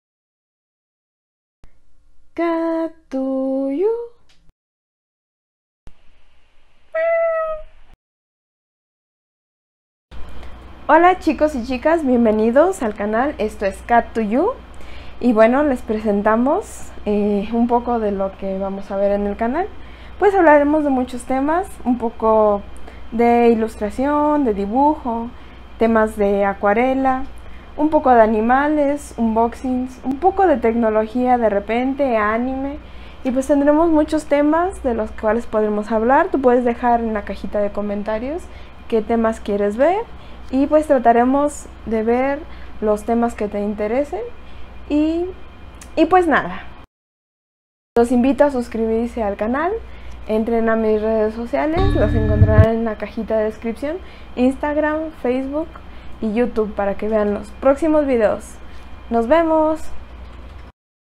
¡Bienvenidos! | Conoce un poco de lo que tendremos en el canal para ti.
Este video solo es un pequeño pre-view de las cosas que veremos más adelante en el canal.
¡Espera pronto nuestros siguientes videos!